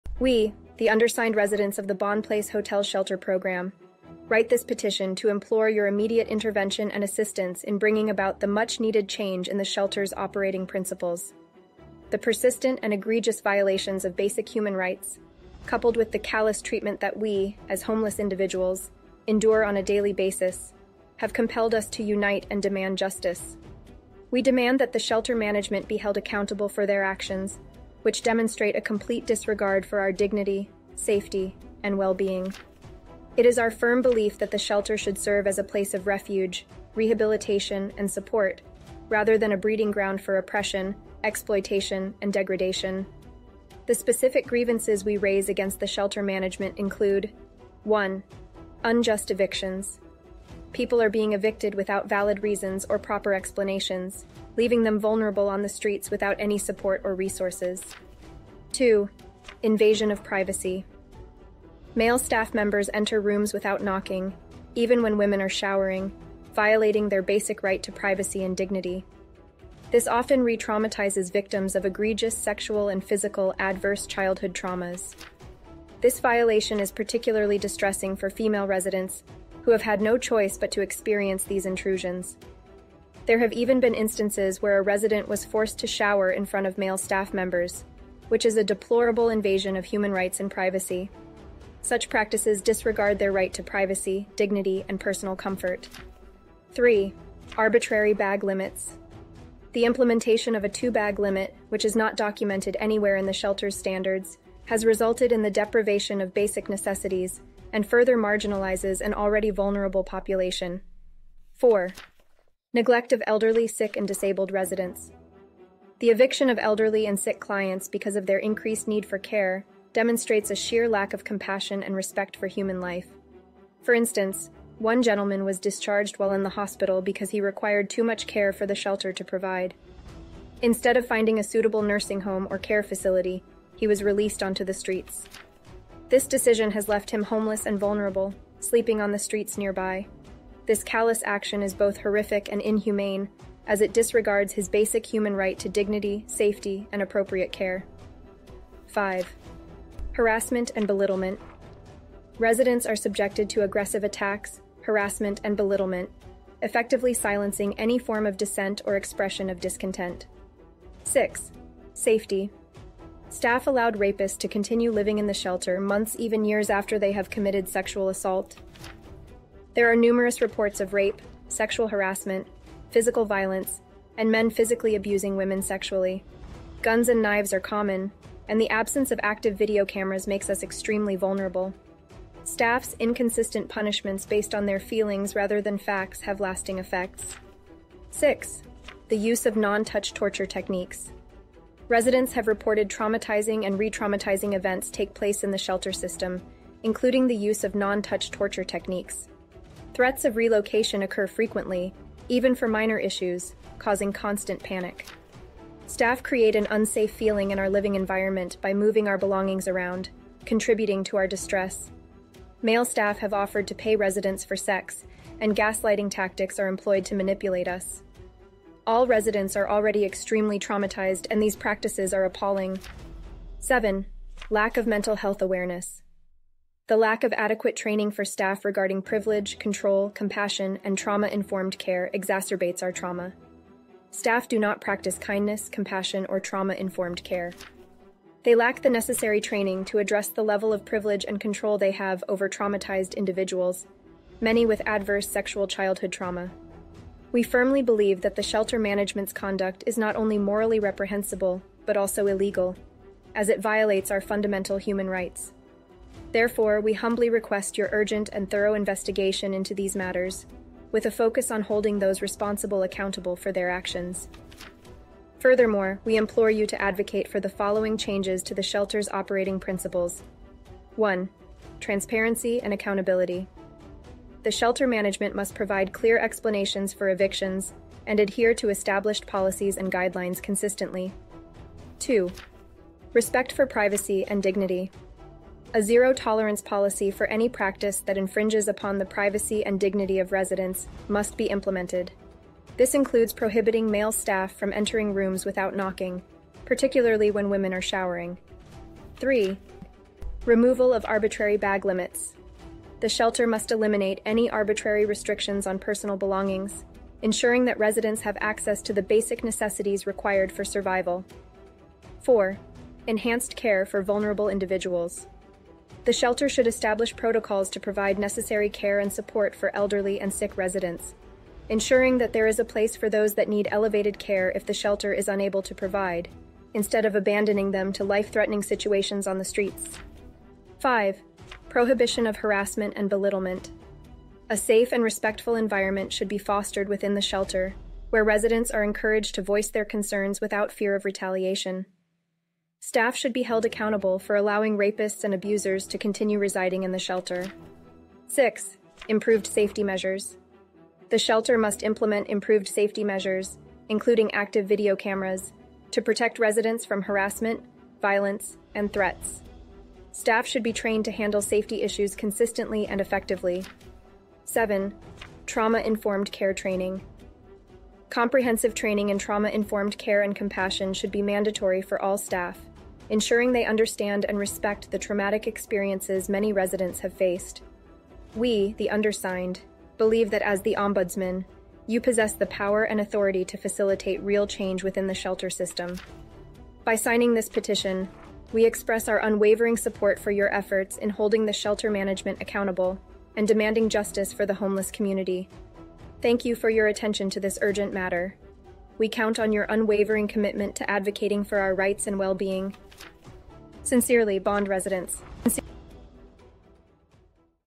BOND PLACE- Petition Addressed to Toronto Ombudsman!
The the Bond Shelter Hotel Program system survivors unit! After years of injustice! we demand shelter management be held accountable for actions against residents!

Homeless people matter although we look rough it take tenacity to live as a homeless person! resilience and how to take it on the chin. remember the saying "Don't hate the player, hate the Game!" its so true. lets change the game to favor humans. we are gods in our own right! l

rape, privacy invasions, being kept in a state of fear, and no adequate training or mental health awareness, unjust evictions just to name a few violations!

"It Means A Lot to Care"